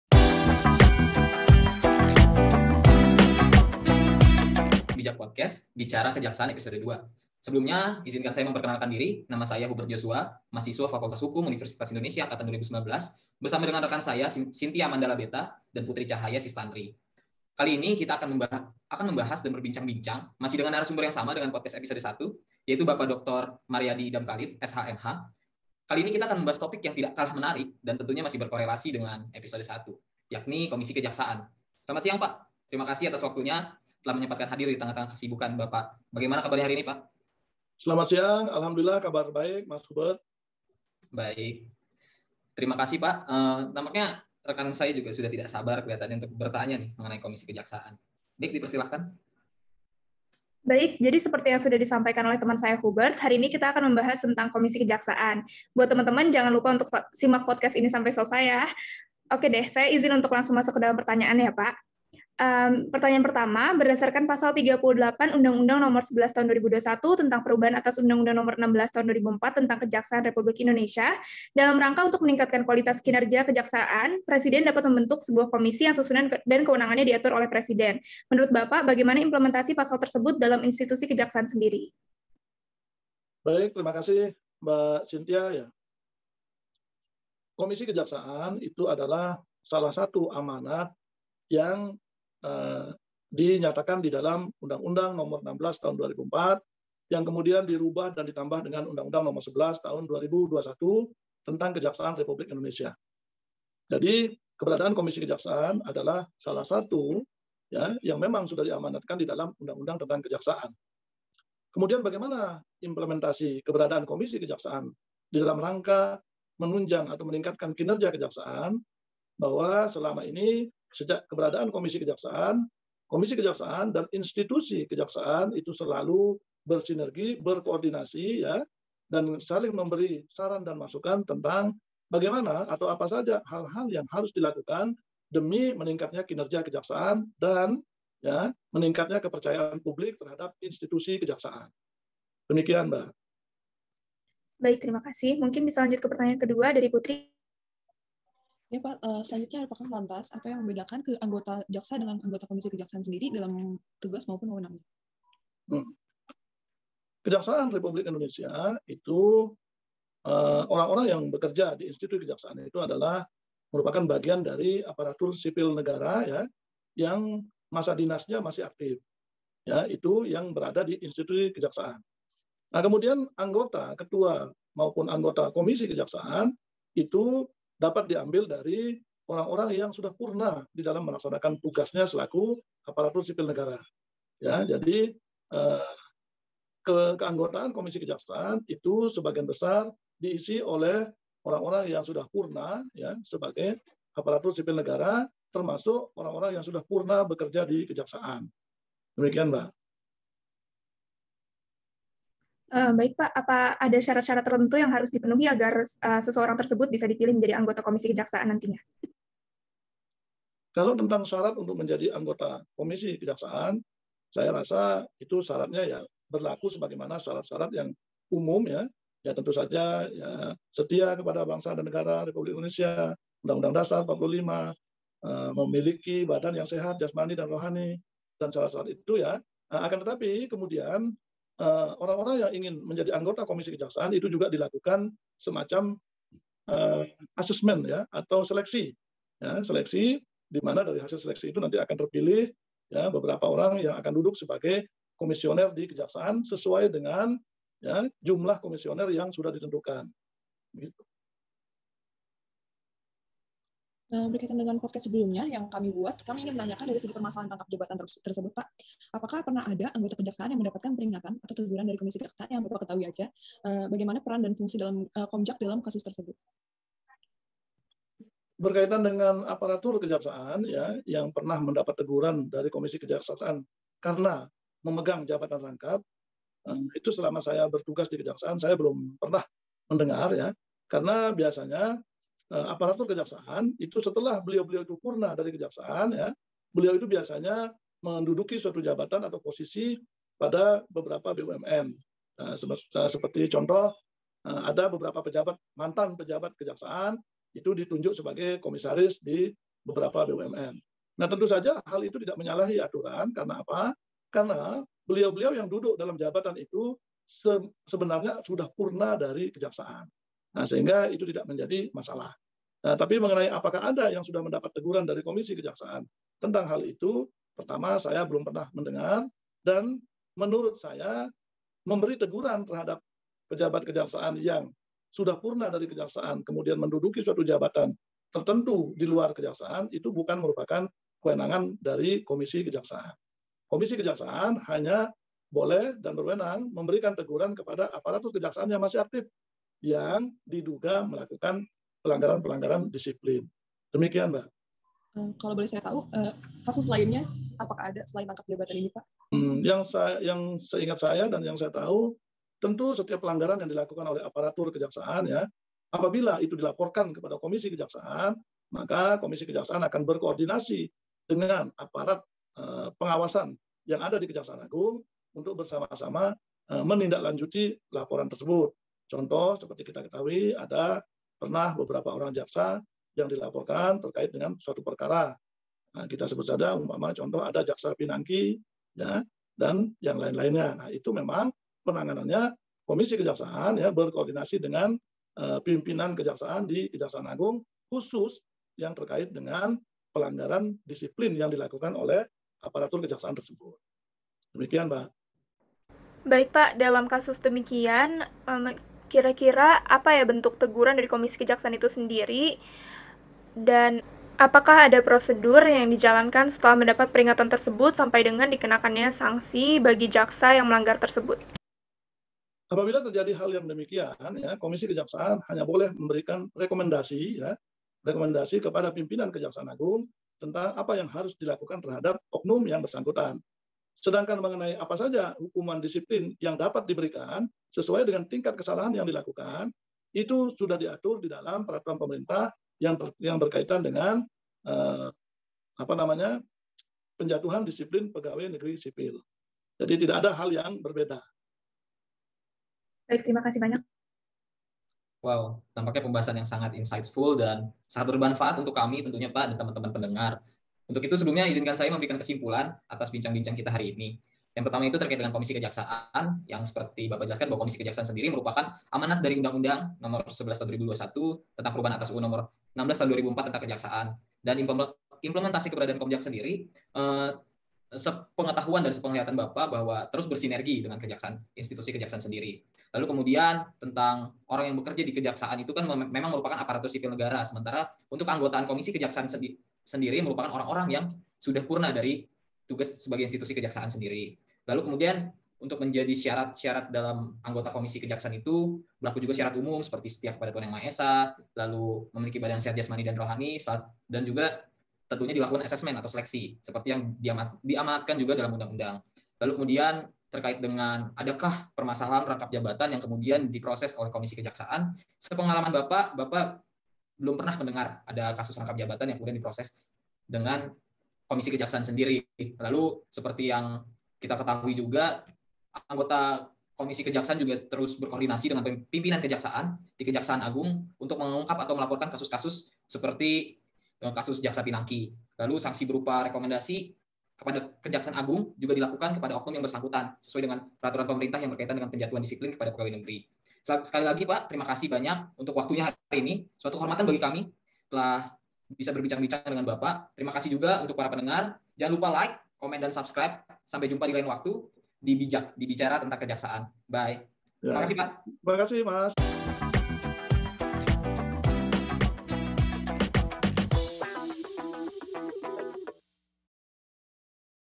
BIJAK Episode #2 Komisi Kejaksaan bersama Dr Maryadi Idham Khalid
BIJAK, Bicara Kejaksaan!

Halo teman-teman! Selamat datang di Podcast BIJAK Bicara Kejaksaan! Perkenalkan, kami dari Kelompok 11 kelas Etika dan Tanggung Jawab Profesi Hukum B Program Paralel. Di episode 2 kali ini masih dengan narasumber yang sama dengan podcast episode 1 yaitu Bapak Dr. Maryadi Idham Khalid S.H., M.H.  Kita akan membahas topik yang tidak kalah menarik dan tentunya ini lanjutan langsung dari episode 1 lo! Penasaran kan lanjutannya bagiamana! Yuk langsung dengerin aja!  Putri, Hubert dan Cintya akan memandu kalian membahas tentang komisi kejaksaan!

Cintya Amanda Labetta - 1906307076
Hubert Josua Paruhum P - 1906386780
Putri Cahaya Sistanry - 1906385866